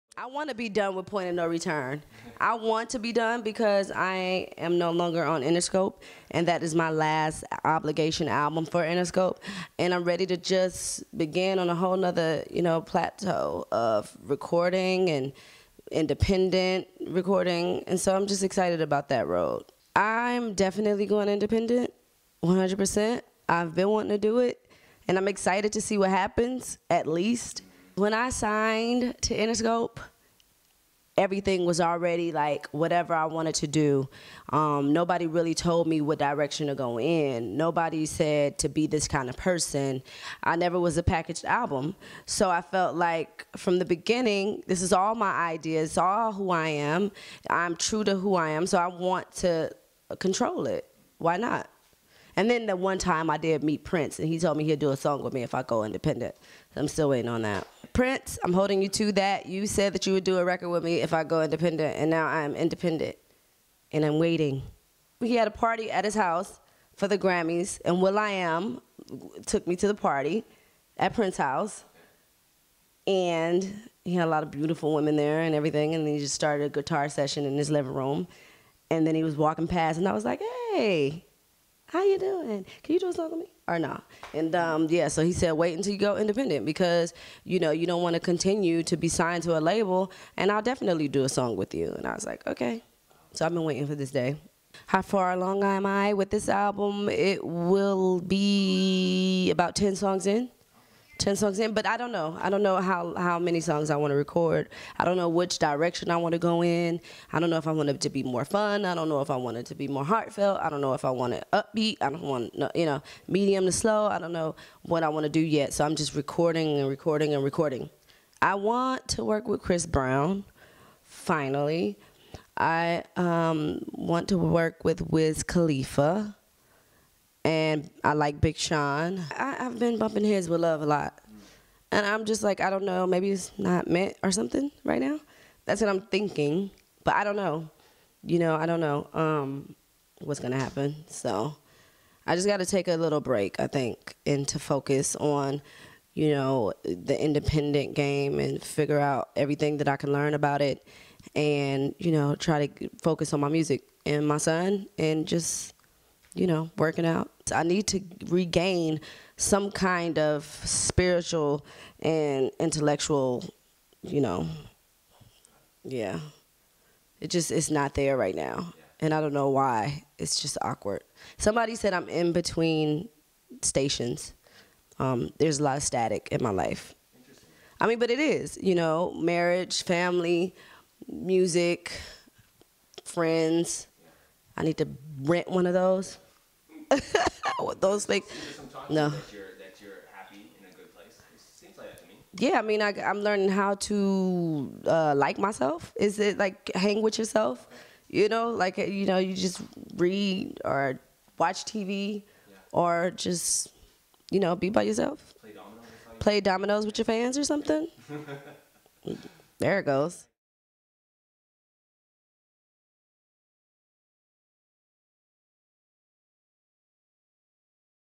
Keyshia Cole Goes Independent, Vows Celibacy
Keyshia Cole takes a vow of celibacy and reveals how Prince influenced her decision to become an independent artist.